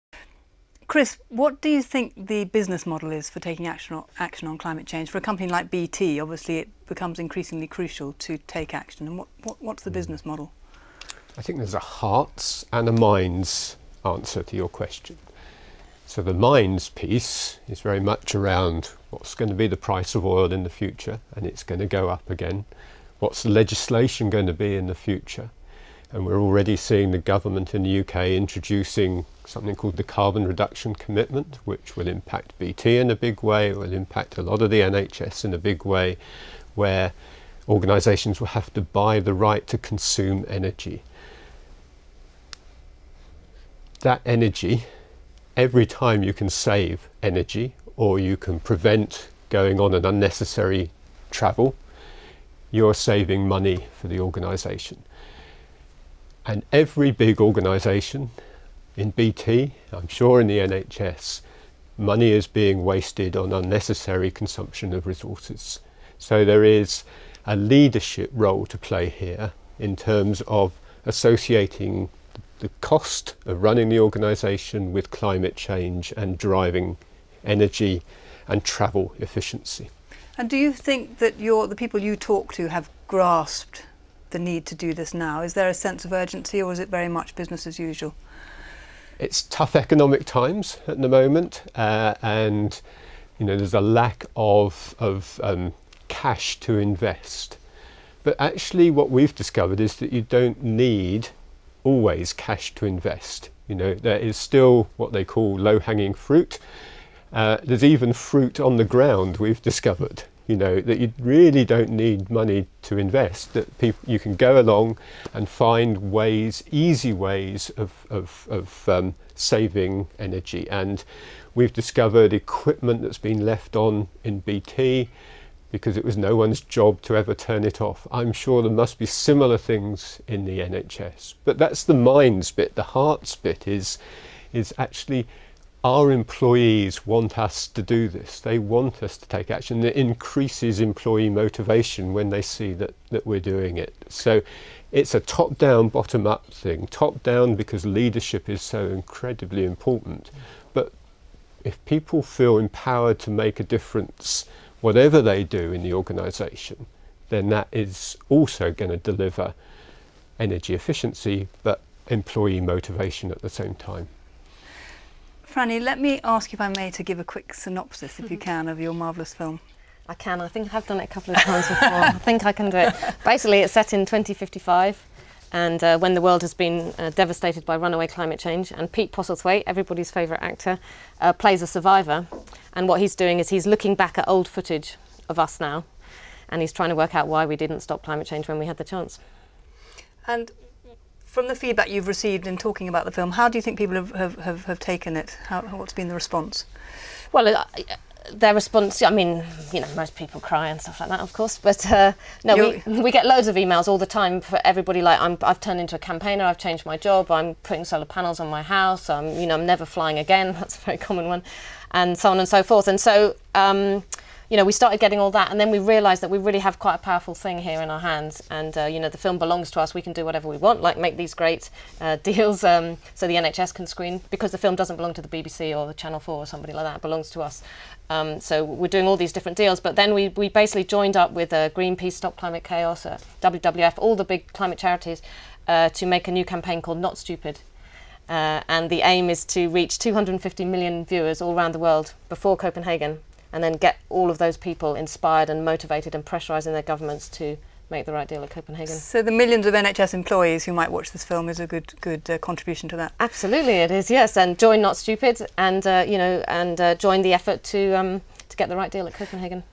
Part 4 of 6 Why should the NHS take climate change seriously? The environment and health.
A panel discussion recorded on the 7th July 2009 chaired by Fiona Godlee, editor in chief of the BMJ.

High level Doctors and leaders in sustainability give their comments on why the NHS should take climate change seriously and give their views on the  environmental film The Age of Stupid and why NHS staff should see it.  The panel consider why the NHS must reduce its carbon footprint and how reducing it will improve healthcare.

Taking part are:
Fiona Godlee, editor in chief of the BMJ;  Franny Armstrong, Director of The Age of Stupid;  Professor Hugh Montgomery, University College London; Dr David Colin-Thomé, Dept of Health; Dr Angela Raffle, Public Health Doctor and expert in Sustainability and Peak Oil;  Chris Tuppen, Chief Sustainability Officer at BT.